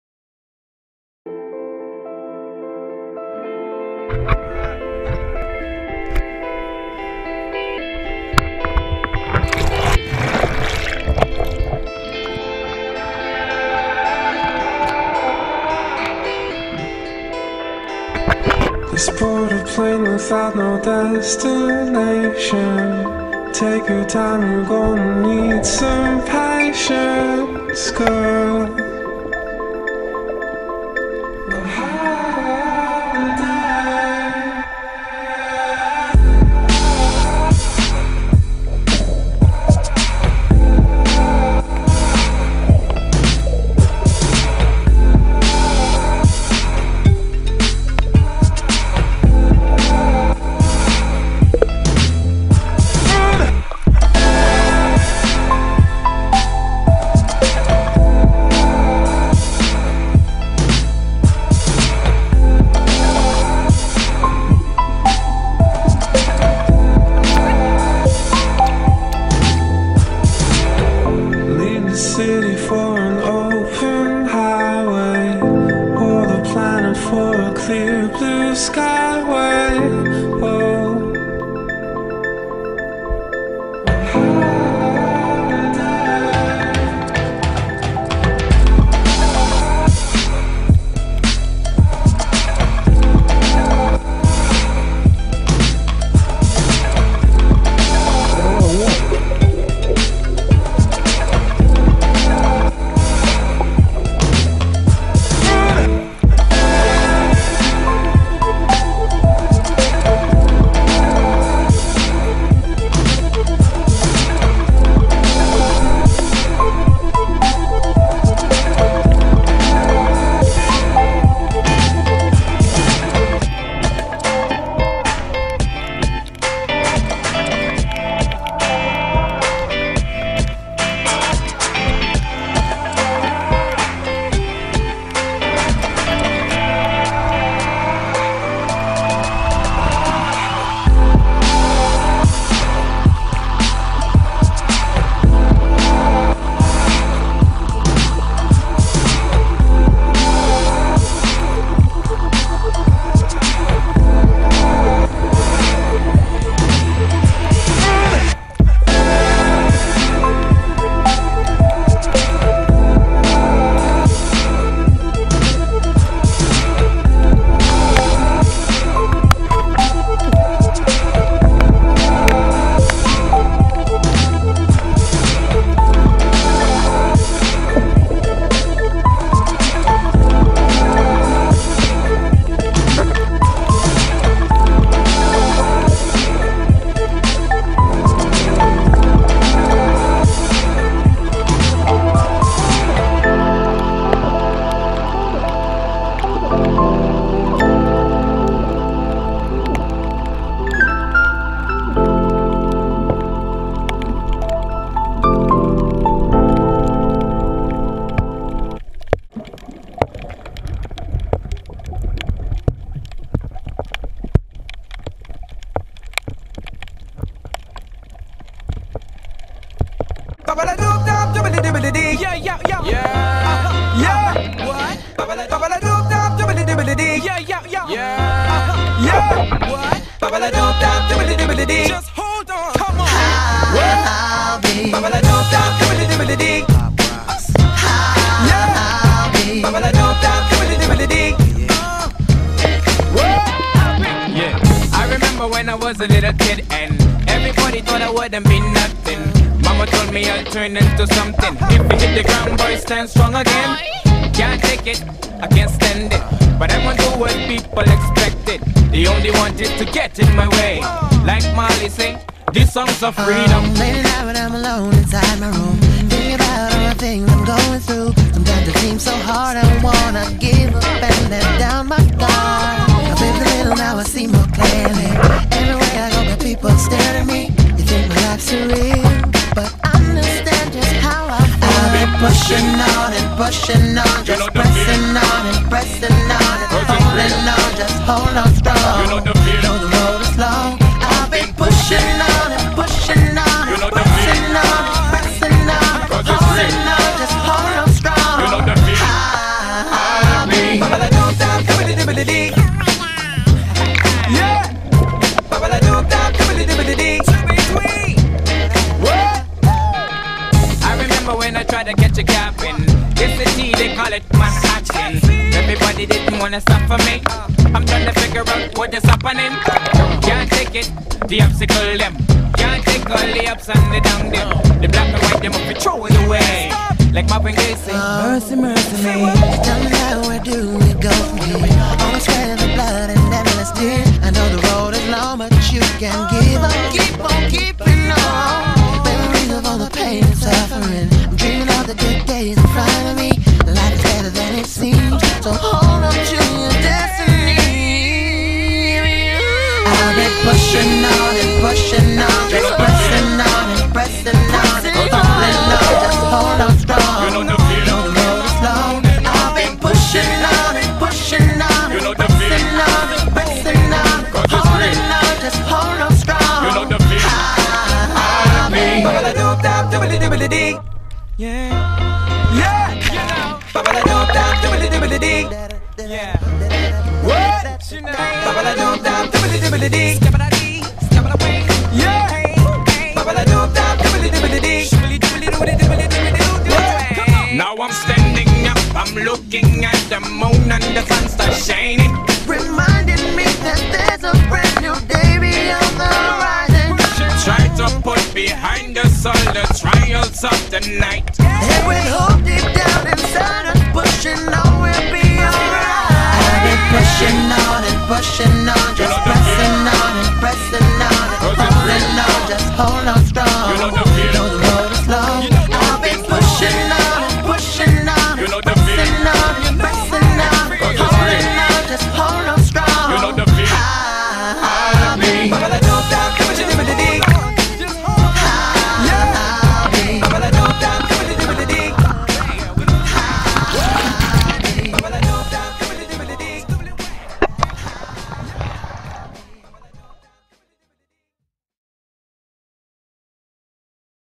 MAAC club caves and snorkeling, 2013
snorkeling through caves and just enjoying our sunday mornings at the MAAC club. enjoy the video and there will be more fishing and spearfishing videos uploaded in the future. enjoy :)